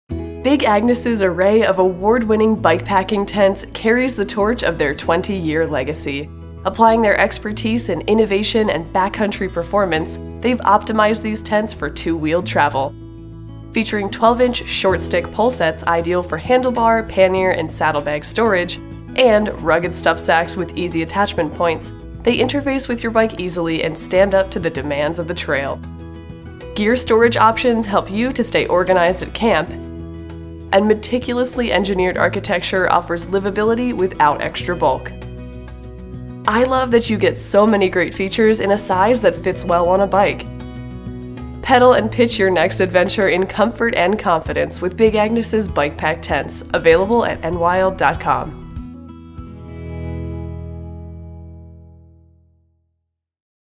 Bikepacking with Big Agnes
For 20 years, Big Agnes has been building innovative gear designed for comfort in the backcountry, whether you're on two feet or two wheels. Their award-winning bikepacking tents feature a shortened 12" Shortstik Poleset for optimal handlebar, pannier & saddlebag storage, plus a burly compression sack with integrated daisy chain webbing for easy bike attachment. Spacious sleeping area and maximized gear storage options mean you can happily pedal and pitch your next adventure in comfort. Pick up a Big Agnes Bikepacking Tent for your next two-wheeled trip right at Enwild